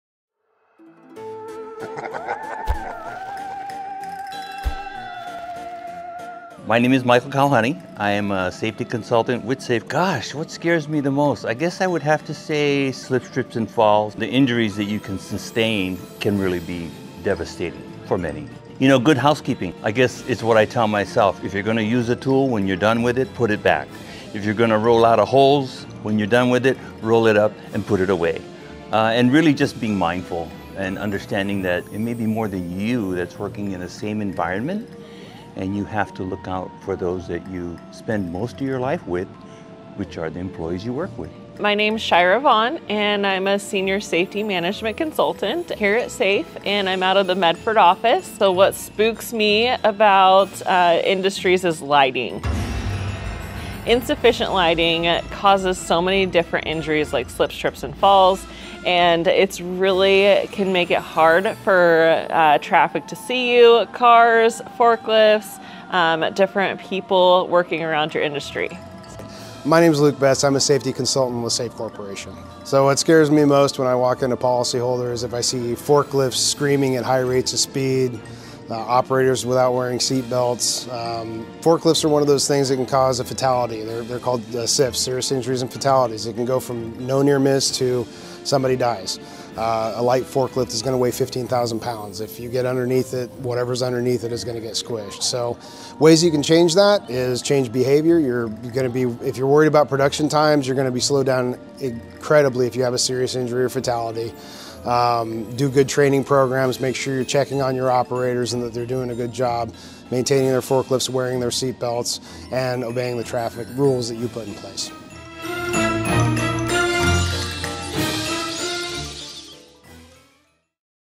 Safety pros share what spooks them the most
We asked our safety consultants what spooks them the most when they walk into a business. Don't let these spookies get in the way of your safety